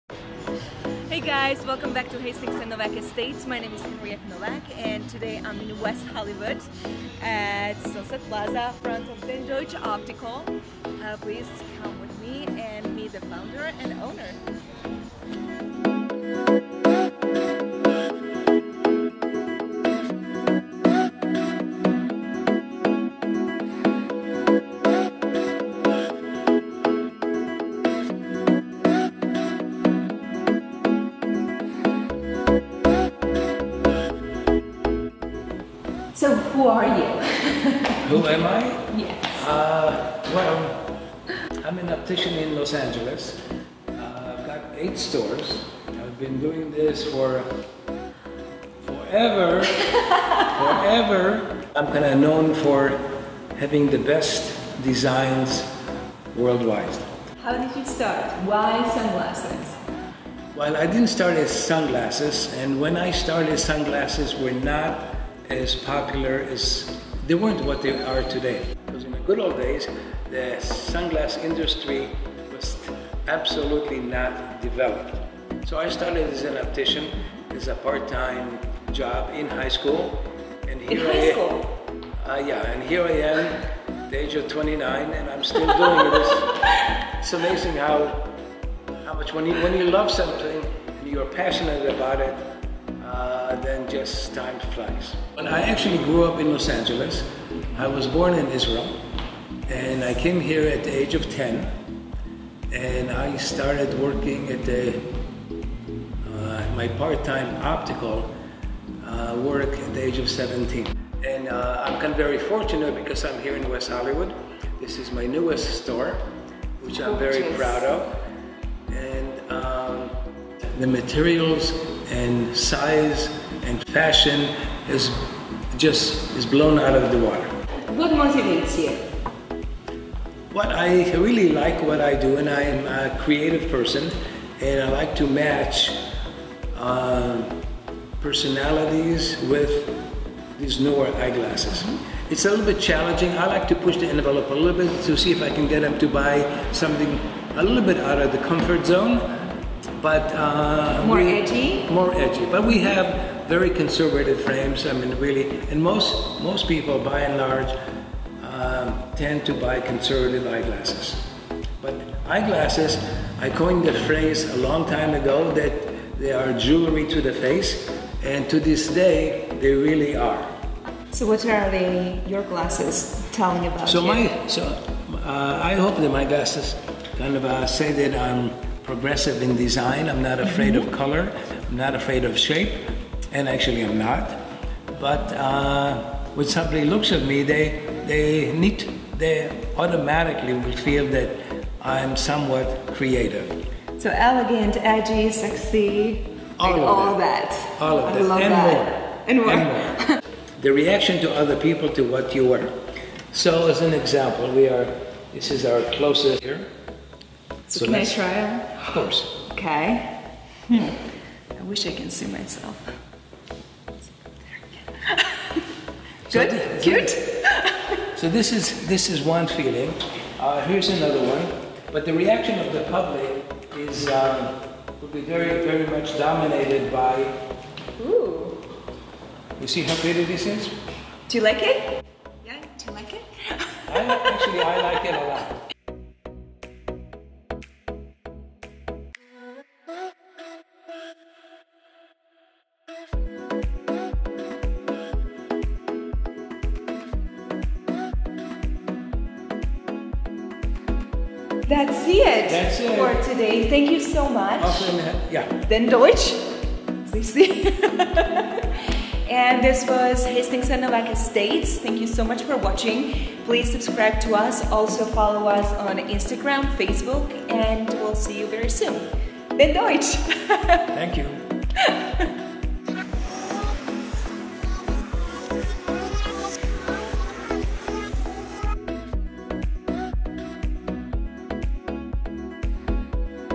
Dan Deutsch Optical Outlook with Henriett Novak
Dan Deutsch has been styling the who’s who of Los Angeles since he first opened his doors. He has taken the simple motto, that “glasses are jewelry to the face and the conviction that: “eyewear defines who you are” from his youthful beginnings all the way to his role today, directing the largest independent chain of retail eyewear stores in the Los Angeles area. He serves as a frequent consultant not only to film and television studios but also to eyewear designers and manufacturers.